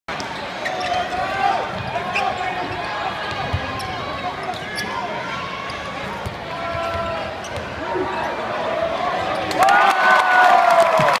AWESOME BASKET BY university of texas-arlington
college basketball`s opening match between university of texas-arlington vs southern texas university. UTA won 89-82 in a closely fought match starting off their season with a promising win at their home in college park center,arlington